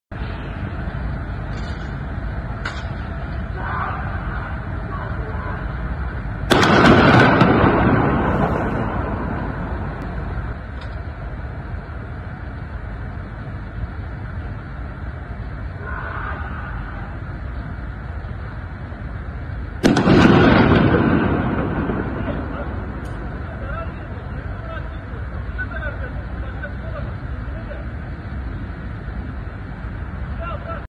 Ege Üniversitesinden randevu alamıyorum deyip tüfekle rastgele ateş açtı (2)
İzmir’in Bayraklı ilçesinde, Ege Üniversitesi Tıp Fakültesi Hastanesinden randevu alamadığını ileri süren bir genç, elindeki pompalı tüfekle rastgele ateş etti. Özel Harekat ekiplerinin de sevk edildiği olayda uzun uğraşlar sonucu şahıs, tüfeği yere bırakarak ekiplere teslim oldu.